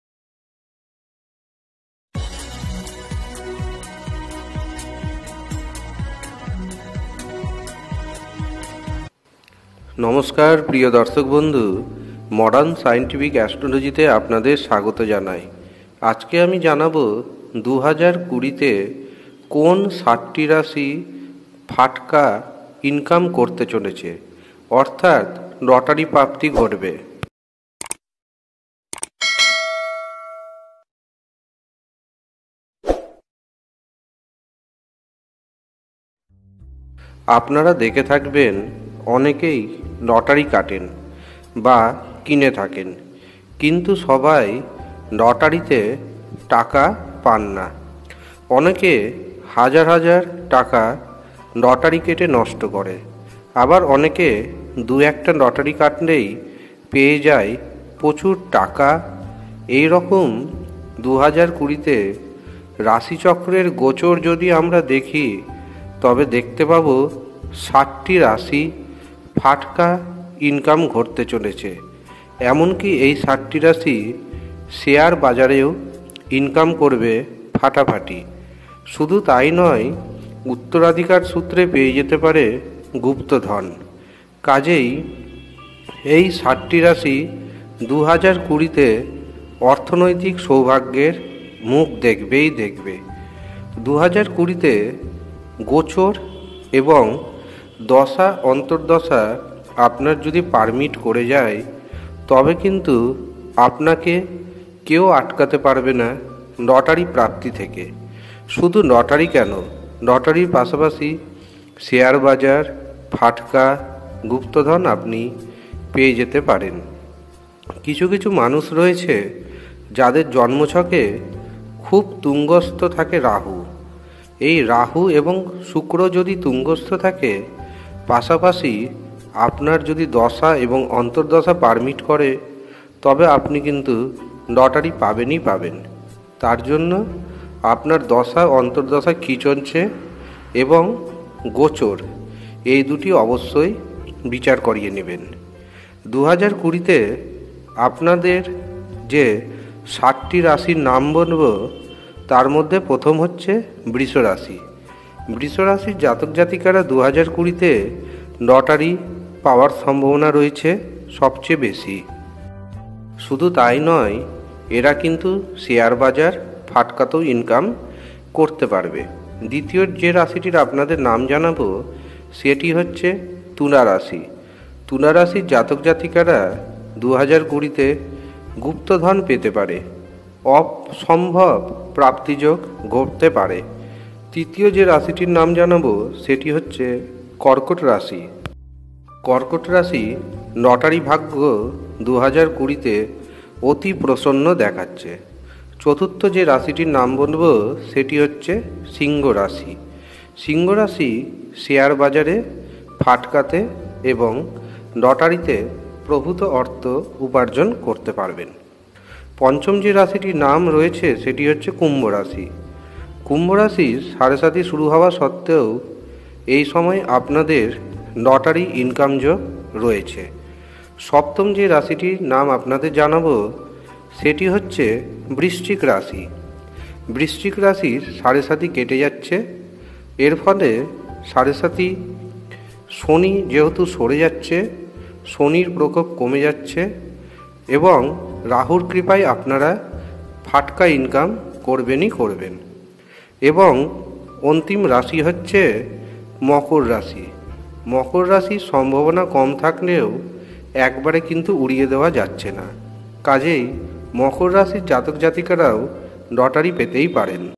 2020 তে লটারি প্রাপ্তির যোগ রয়েছে এই 7 টি রাশির।লটারি2020
2020 তে লটারি প্রাপ্তির যোগ রয়েছে এই 7 টি রাশির।  ভিডিওটি সম্পূর্ণ টি দেখে জেনে নিন কারা কারা 2020 তে লটারির মাধ্যমে অর্থ উপার্জন করতে পারবেন ধন্যবাদ।